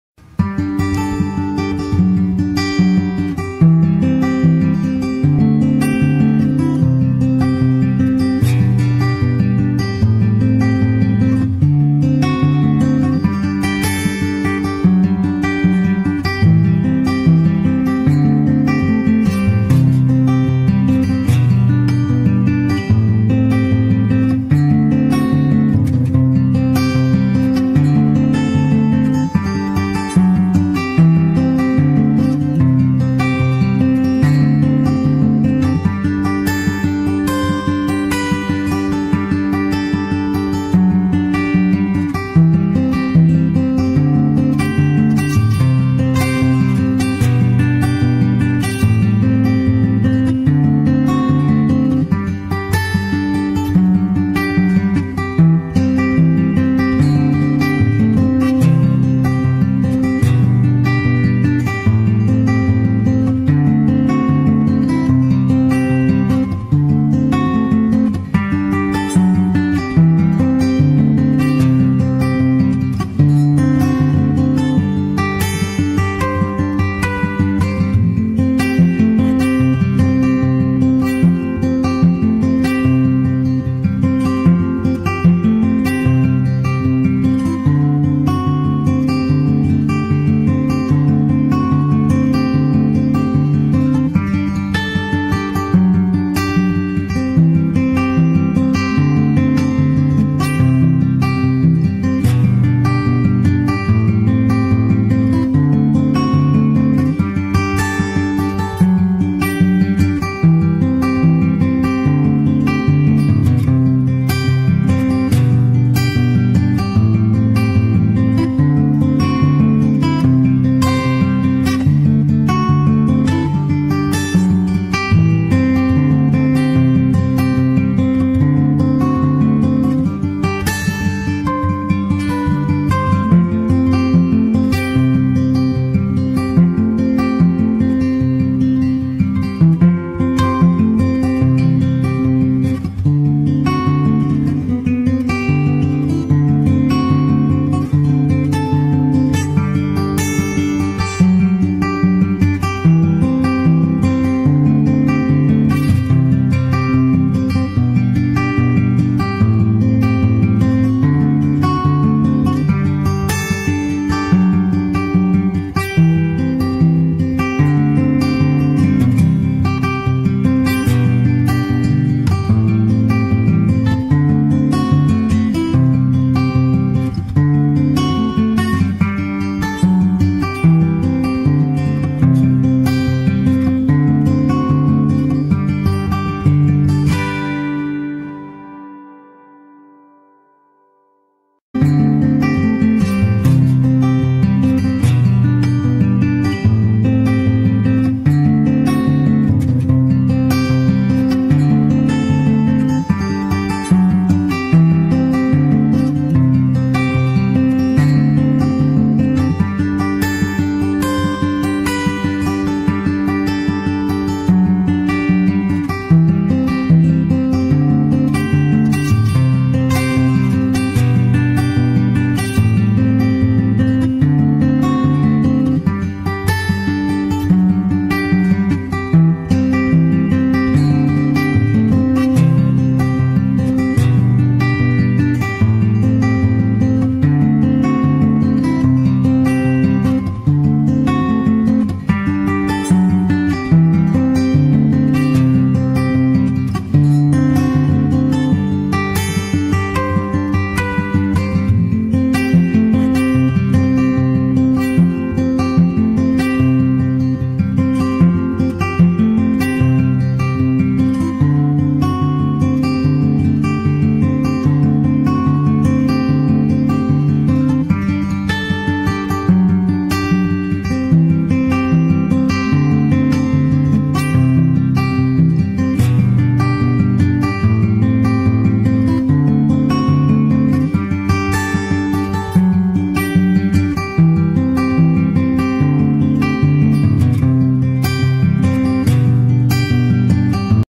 Relaxing with Beutyfull Nature & Animals [loffi Calming Sounds ] Music 🎶for Relaxation
Wonderful planet Earth 🌎 : Beutyfull Animal's  & Nature Scenery

Enjoy the Beautiful Wild Life Footage With Beutyfull Calming Music

Keep Supporting Keep Mediating

Just Relax - Mv